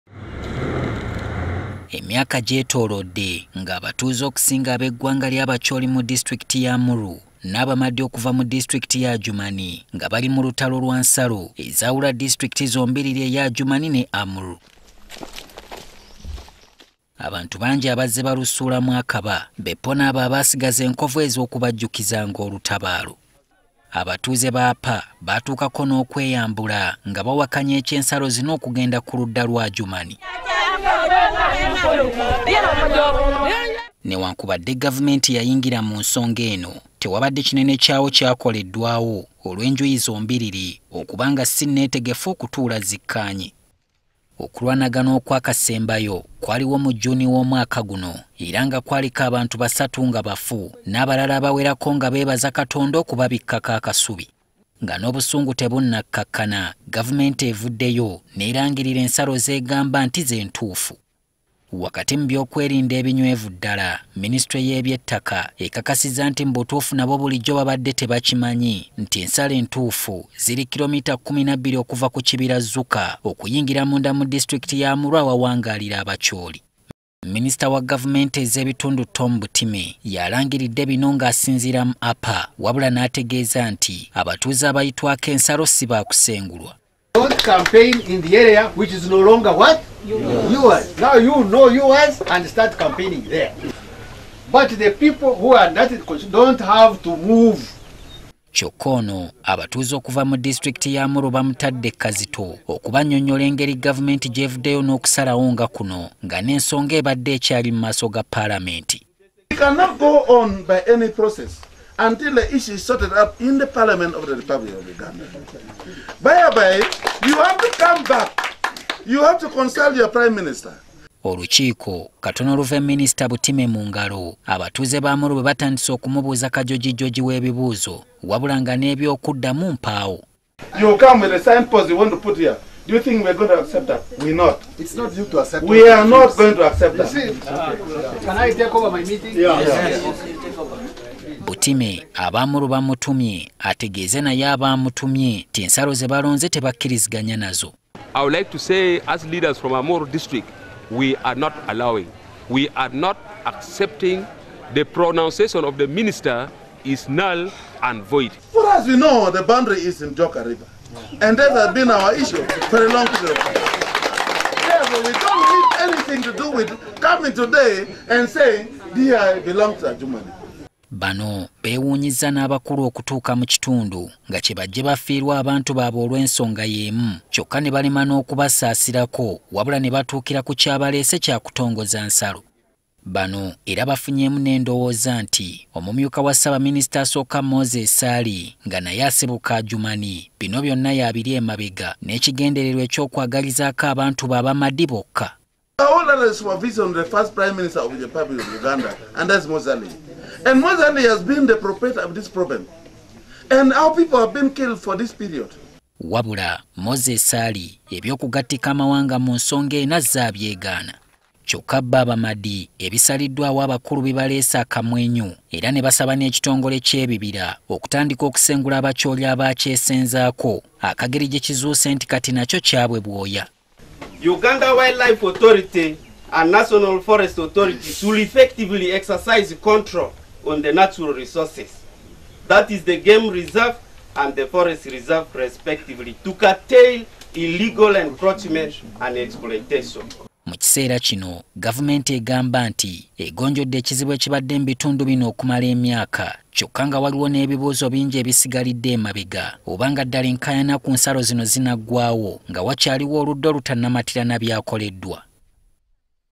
Abatuuze b'e Amuru bawakanyizza engeri gavumenti gy'eyawuddemu Adjumani ne Amuru
Abatuuze mu district y’e Amuru bawakanyizza engeri gavumenti gyeyayawuddemu ensalo wakati waabwe ne Adjumani ezirudde nga ziriko akabuuza.

Abakulu okuva  mu ministry y’eby’ettaka baasazeewo  nti ensalo eyawula Amuru ne Adjumani eri kilo mita 12 okuva ku kibira Zuka okudda mu Amuru.

Minisiter wa gavumenti ez’ebitundu Tom Butime nga abatuuze baamutegezeezza addeyo mu bakama be abategeeze nga kino bwekitagenda kuleetawo mirembe gya nkomeredde.